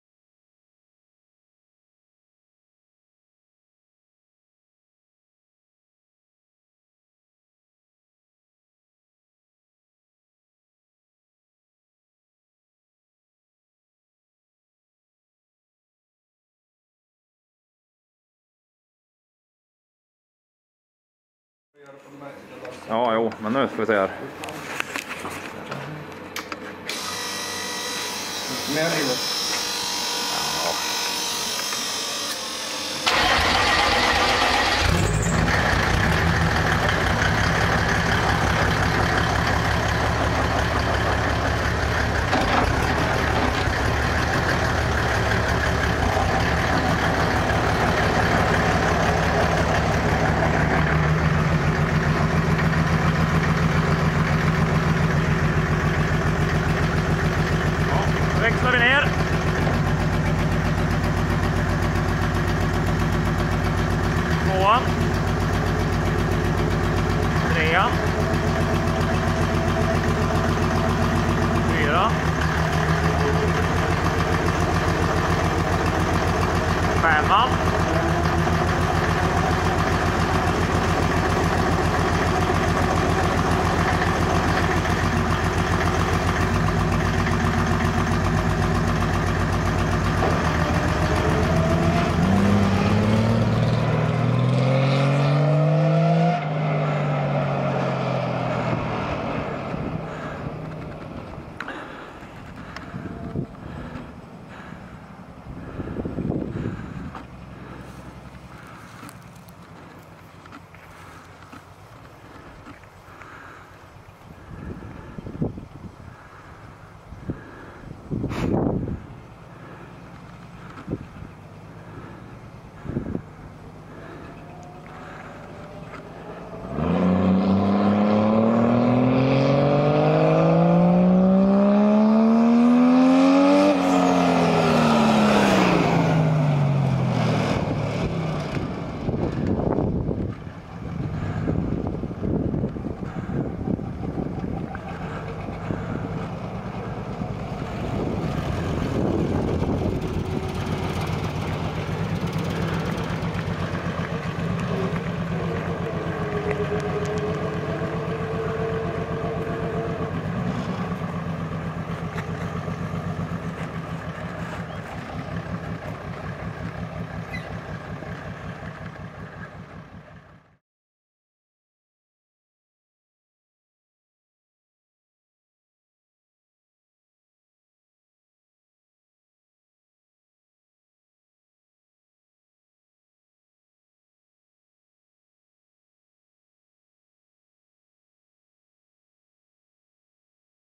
First test of OM606 + NAG1 in Olle and Lasse Andersson's min
First test runt of Olle And Lasse Andersson's mini-puller with new gearbox control system.

The gearbox is a NAG1 (722.6) electro-hydraulic "automatic", and has to be electronically operated in order to work. More info in the clip...